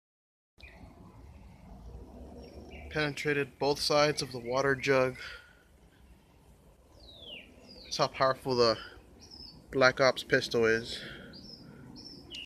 penetration shot results from the black ops air pistol 4.5mm steel bb
the black ops 4.5 mm air handgun penetrated both sides of the water jug filled with water at about 17 feet pretty powerful for a air handgun that's only rated 430 fps